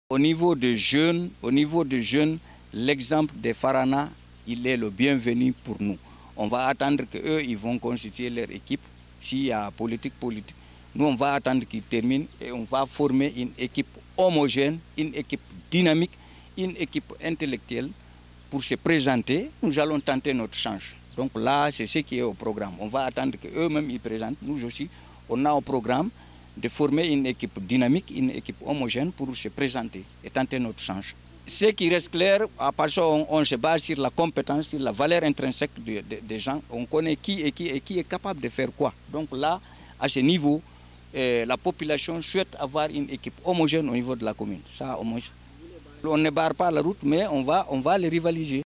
Élections communales : des jeunes de Lelouma présenteront une liste indépendante
Des jeunes de la préfecture de Lelouma située à plus soixante kilomètres du chef lieu de la région administrative de Labé envisagent de rivaliser les politiques lors des prochaines élections élections prévues au mois de février 2017.